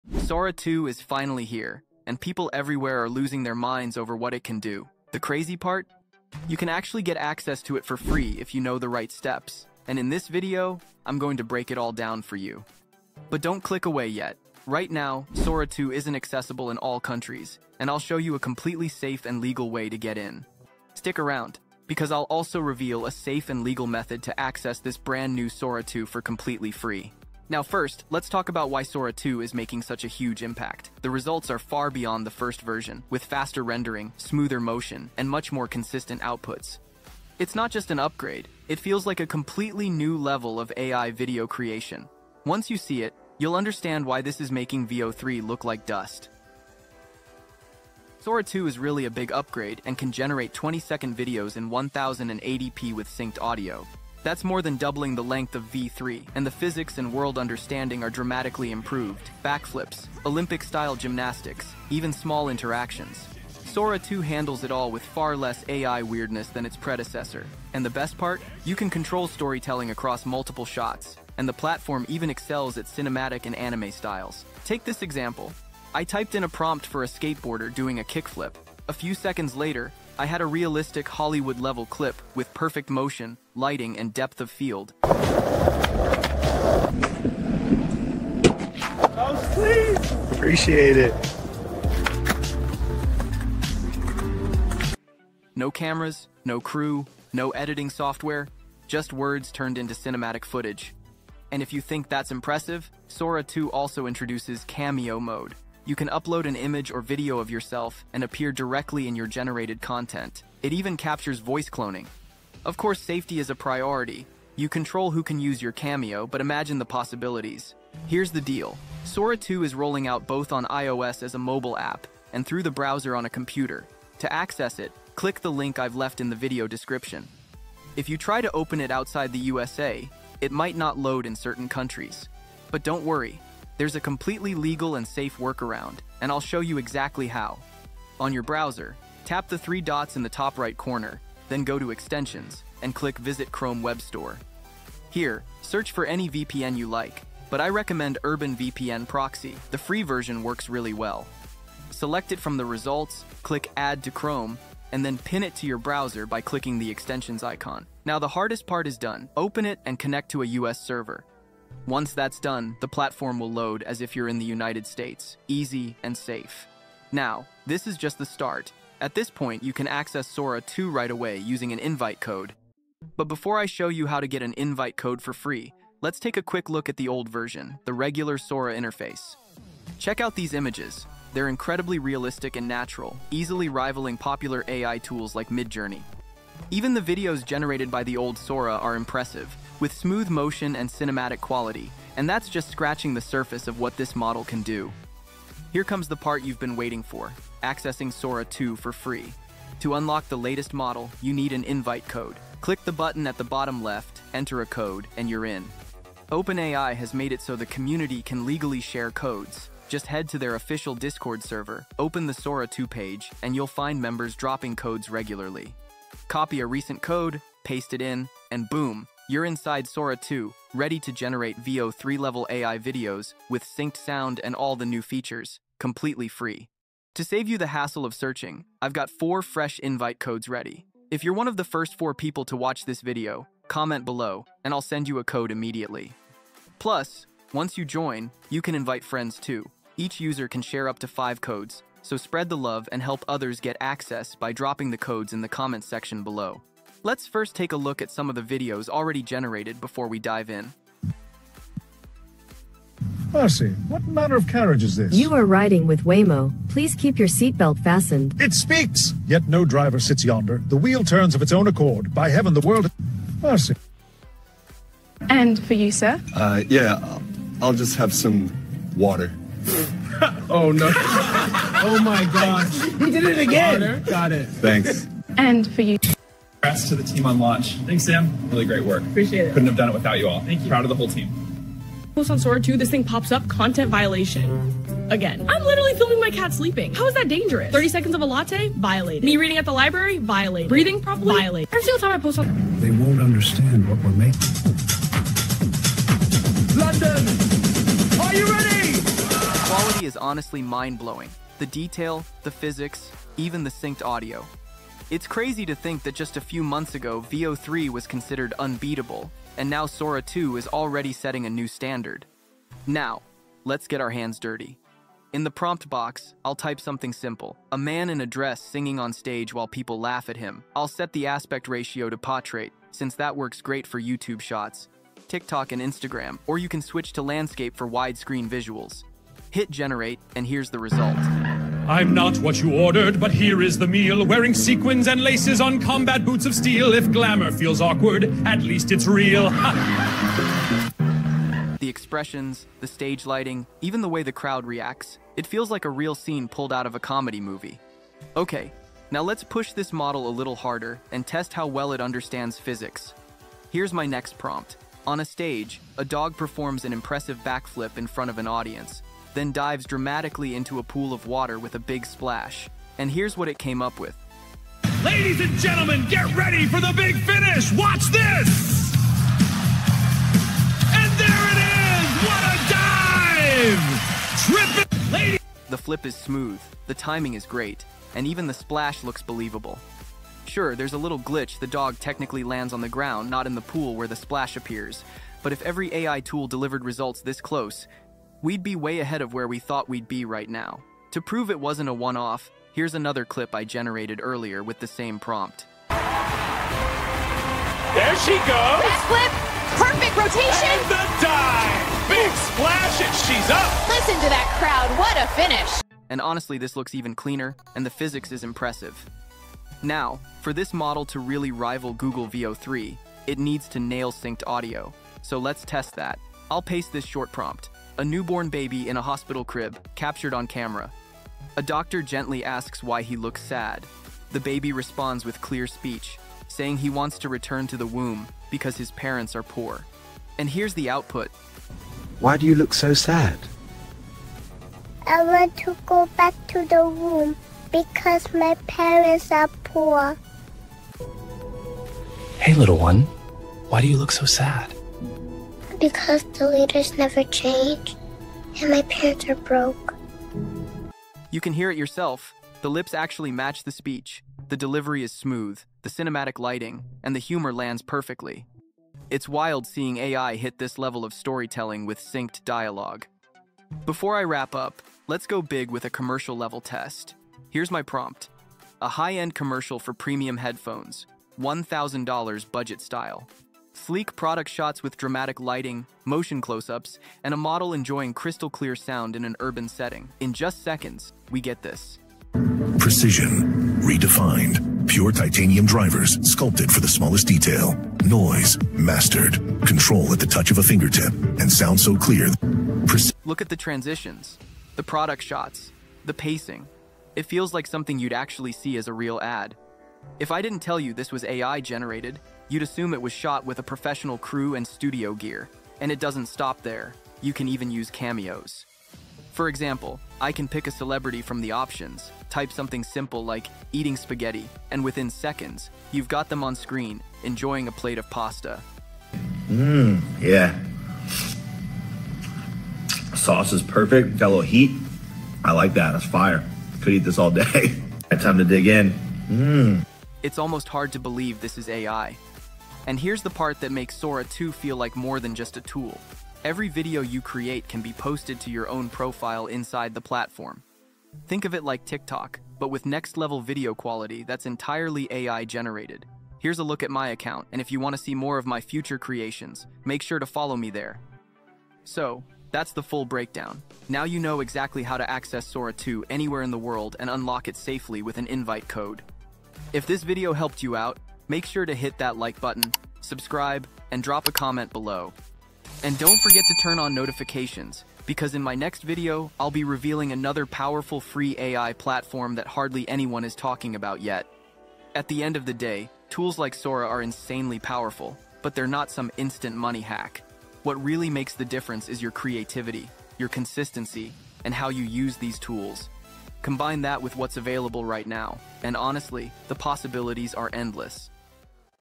How to Get FREE Access to Sora 2 — (Worldwide through Invite Codes)
How to Get FREE Access to Sora 2 — (Invite Codes & Step-by-Step). In this video, I show you exactly how to access Sora 2 for free, unlock it with invite codes, and safely use it anywhere in the world. Watch this step-by-step demo to see how to generate cinematic AI videos with synced audio using Sora 2.